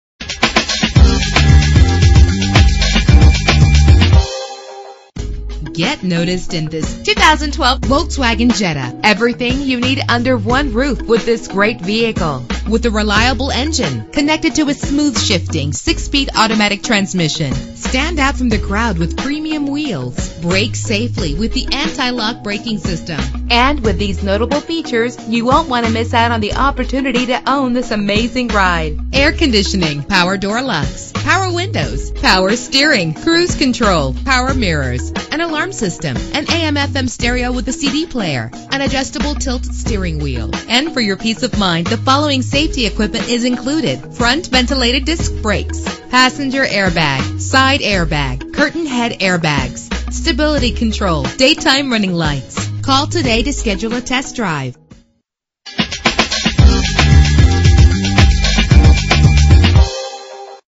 2012 Volkswagen Jetta - Atlanta GA
Year: 2012
Make: Volkswagen
Model: Jetta
Trim: 2.5 SE
Engine: 2.5 liter inline 5 cylinder 20 valve
Transmission: 6-Speed Automatic
Color: Platinum Gray Metallic
Mileage: 0
Address:
5901 Peachtree Industrial Blvd